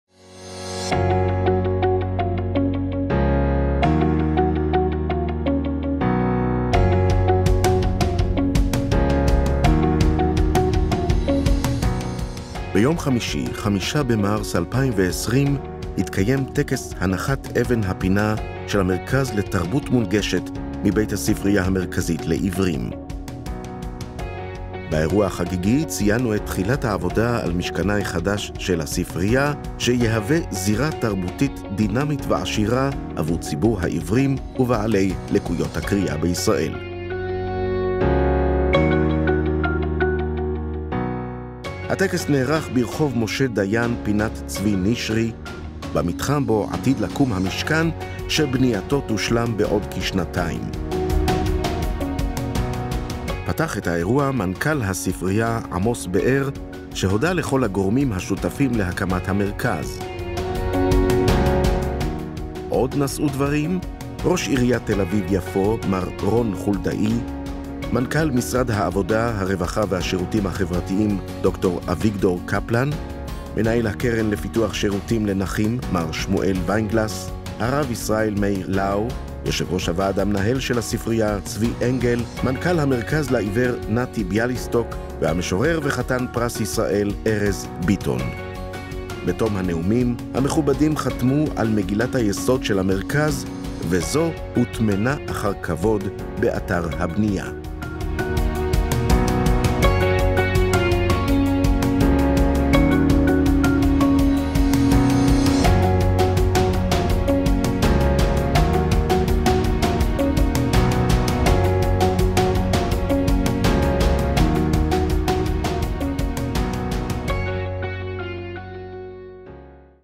הנחת אבן הפינה של "המרכז לתרבות מונגשת"
טקס הנחת אבן הפינה של "המרכז לתרבות מונגשת ע"ש שוורץ רייסמן" מבית הספריה המרכזית לעיוורים נערך ב-5.3.2020 ברחוב משה דיין פינת צבי נשרי בת"א. בטקס החגיגי ציינו את תחילת העבודה על משכנה החדש של הספריה, שיהווה זירה תרבותית דינמית ועשירה עבור ציבור העיוורים ולקויי הקריאה בישראל. המכובדים חתמו על מגילת היסוד של המרכז וזו הוטמנה אחר כבוד באתר הבנייה. הספריה מודה לכל השותפים, המוסדות השונים, התורמים והעוסקים במלאכה שמלווים אותה לאורך כל הדרך.